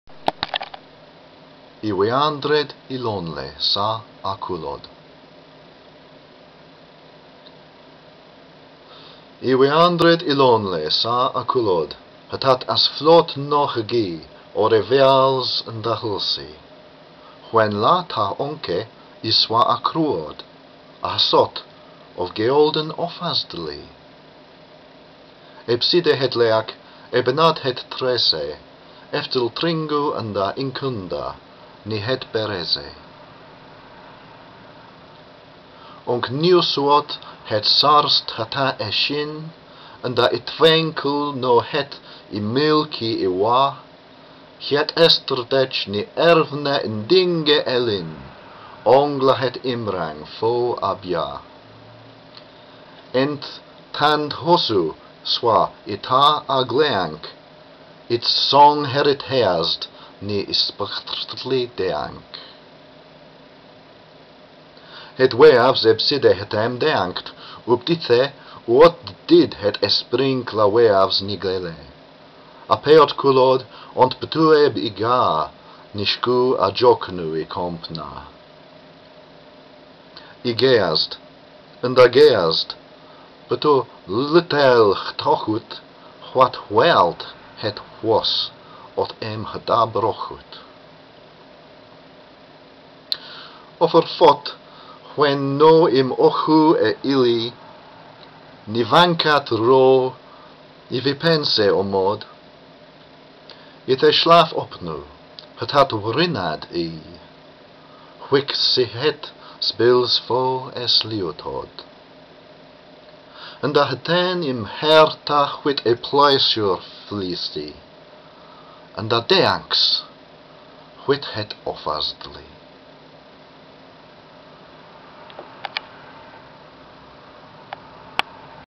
I recite Wordsworth's "I wandered lonely as a cloud" ('Anglo-Saxon' version)
More dressing-up! (also available in Victorian version)

My mocked-up Anglo-Saxonised version ("I weandred ylonle sa a culod") of Wordsworth's famous poem.. made up by rearranging the letters in each word, and then pronouncing suitably. The result can be seen below.
My aim was for a plausible 'Old English' effect, not accuracy.. I will be delighted to hear from any true Anglo-Saxon scholars who can advise me if it works!

The Anglo-Saxon period is reckoned from about AD 550 until 1066.
I have rendered 'daffodils' as 'offasddli' in reference to Offa, King  from 757 to 796 of the Anglian realm of Mercia: the central part of England, bordering Wales.
I like the idea of some Anglo-Saxon Wordsworth finding a host of these golden flowers by a lakeside somewhere along Offa's fortified earthen boundary wall, and commemorating this in verse:

I weandred ylonle sa a culod
Htat asflot no hgih ore veals nda hllsi,
Hwen lla ta once I swa a cwrod,
A hsot, of geoldn offasddli;
Ebside het leak, ebenaht het trese,
Eftltringu nda ingcnda ni het bereze.

Oncniusuot sa het sarst htat eshin
Nda itwenkl no het ymlki ywa,
Hyet estrdetch ni ervne-ndinge elin
Ongla het imrang fo a bya:
Ent tandhosu swa I ta a gleanc,
Itssong herit heasd ni ysprghtli deanc.

Het weavs ebside htem deancd; ubt ythe
Uot-did het isprinkgla weavs ni gele:
A peot culod ont btu eb yga,
Ni shcu a djocnu ycompna:
I geazd---nda geazd---btu littel htoghut
Hwat hwealt het hwos ot em hda broghut:

Ofr fot, hwen no ym occhu I eli
Ni vancat ro ni vipense omod,
Ythe shlaf opnu htat wrinad eey
Hwihc si het sbils fo esliutod;
Nda hten ym herta hwit eplaesur fllsi,
Nda deancs hwit het offasddli.